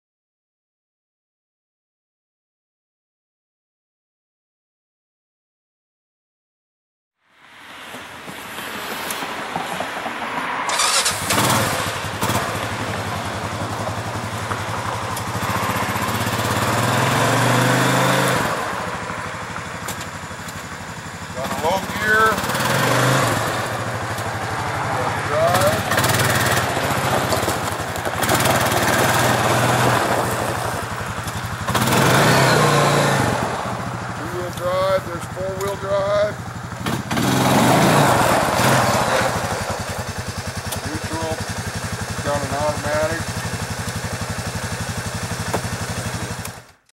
2005 Honda Rubicon ATV for sale | sold at auction December 6, 2012
SOLD! Duffield Construction Auction

2005 Honda Rubicon ATV
1,235 miles on odometer
279 hours on meter
Gas engine
L-D-N-R gears
Four wheel drive
D1, D2, ESP switch
Cargo racks
Head lights
Taillights
Rear toolbox
2" ball hitch
4&#039; Liter Force plow blade
Quick attach mount

It was located in Rapid City, SD, and we sold it to the highest bidder for $4,400.